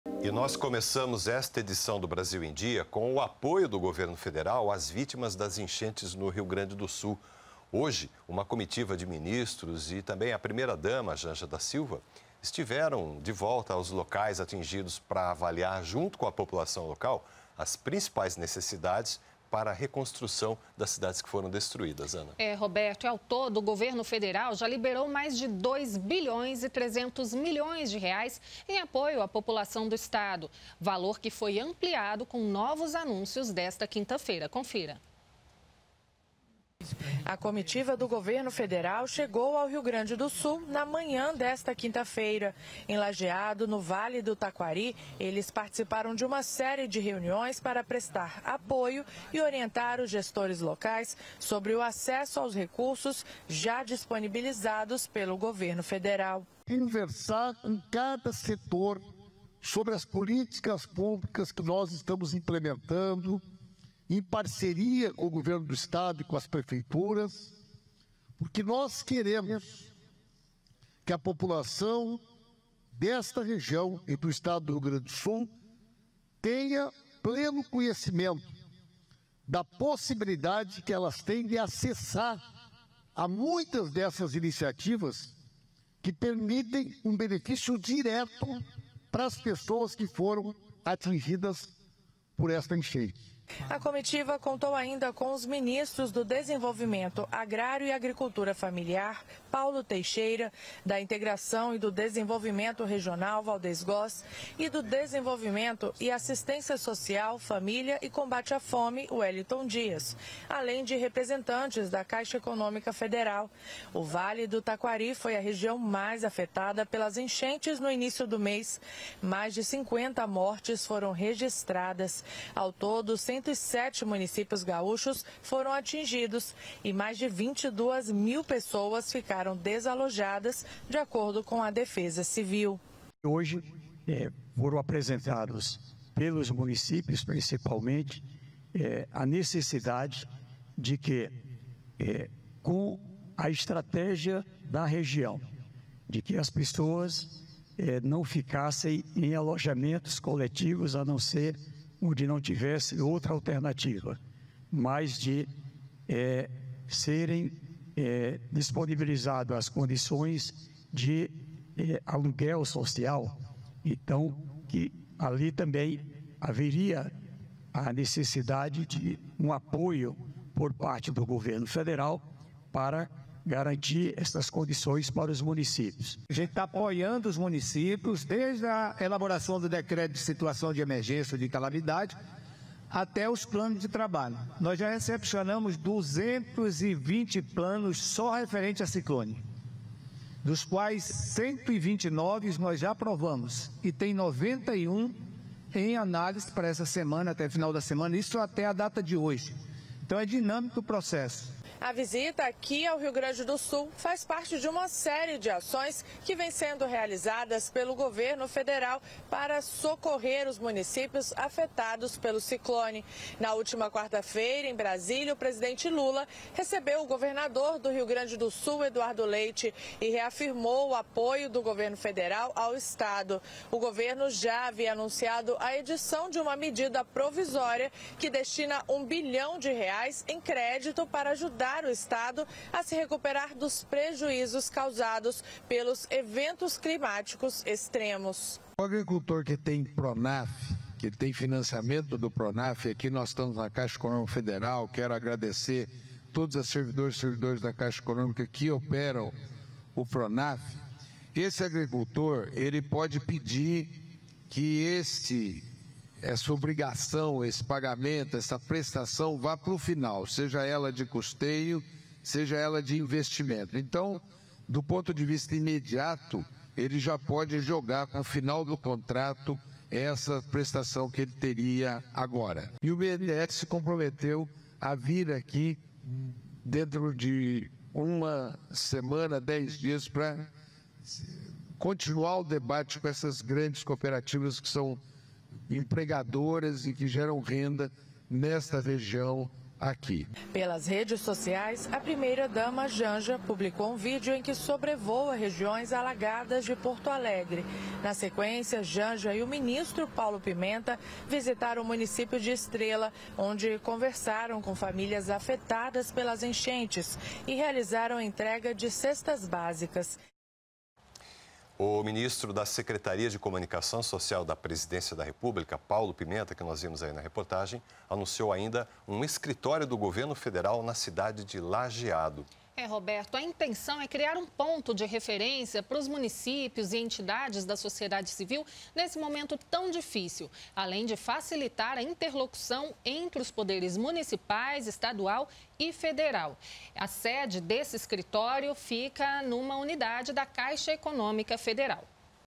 PRIMEIRA-DAMA E MINISTROS VISITAM ÁREAS ATINGIDAS POR CHUVAS NO RIO GRANDE DO SUL
Uma comitiva formada por ministros e pela primeira-dama, Janja da Silva, visitaram a região atingida por enchentes no Rio Grande do Sul para avaliar as principais necessidades. Ao todo, o governo já liberou mais de R$ 2,3 bilhões em apoio à população do estado, valor ampliado com os anúncios desta quinta (29).